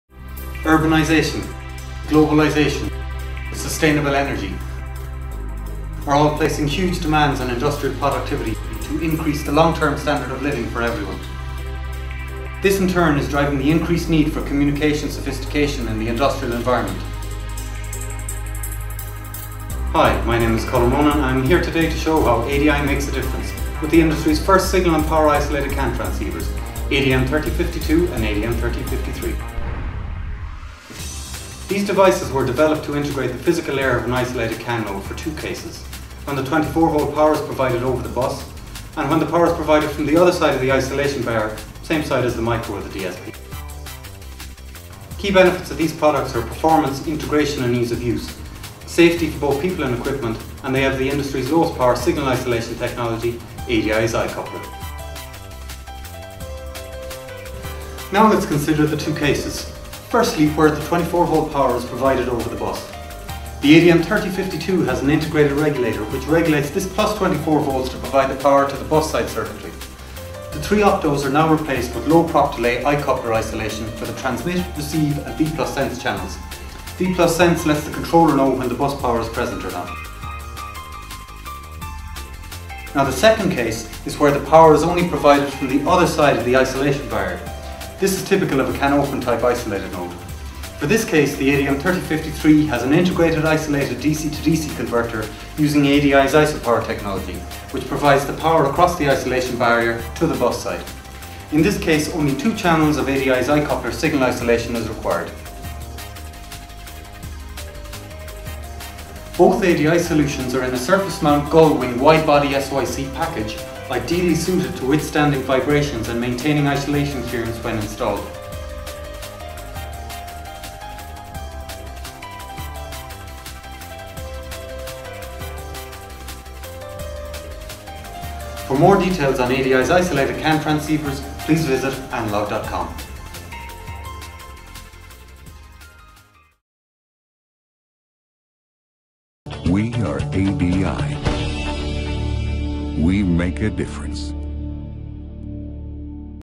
ADM3052 / ADM3053: Industry's First Fully Isolated Industrial CAN Transceivers
The ADM3052 and ADM3053 are isolated CAN physical layer transceivers. The devices employ Analog Devices' iCoupler® digital isolator technology to combine a 3-channel isolator, a CAN transceiver and an LDO regulator into a single package.